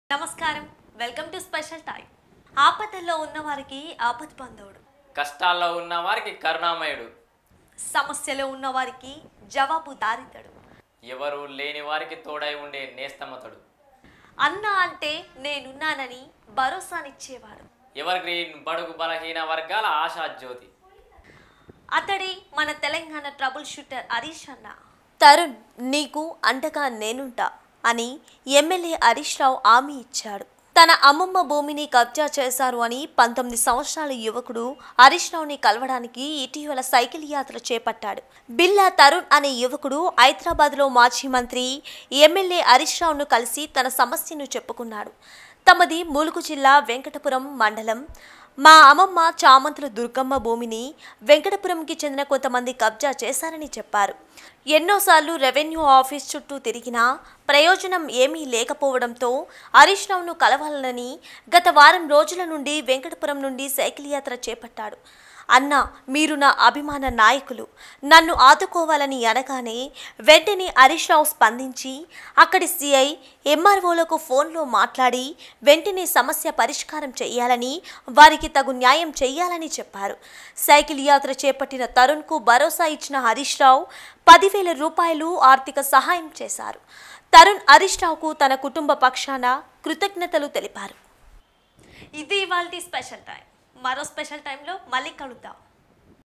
TS24 News తరుణ్.. నీకు అండగా నేనుంటా...! ఎమ్మెల్యే హరీష్ రావు హామీ
తరుణ్.. నీకు అండగా నేనుంటా...! ఎమ్మెల్యే హరీష్ రావు హామీ